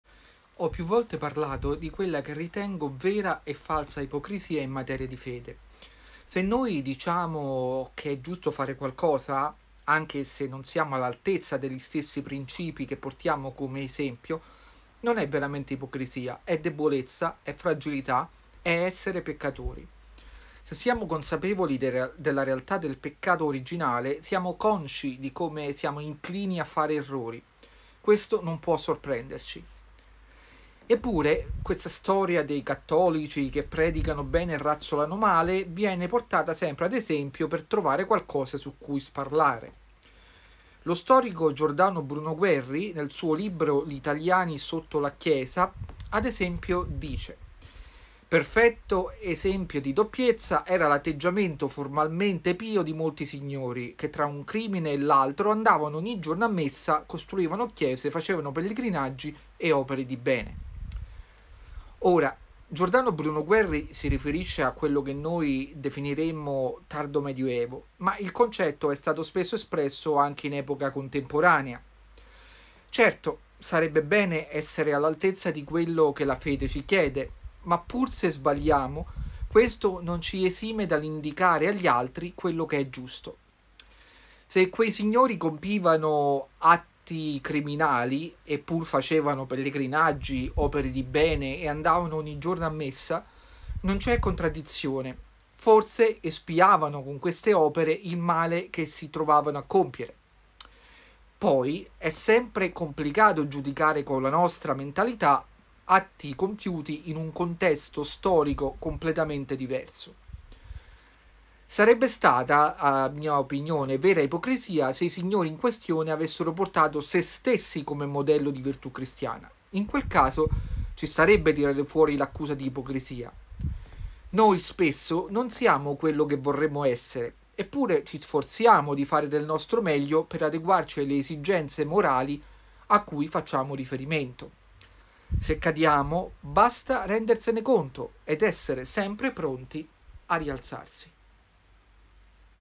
L’opinione. Vera e falsa ipocrisia
A volte i cattolici vengono accusati di ipocrisia perchè la loro vita non si conforma ai princìpi della loro fede. Ma le cose sono più complicate….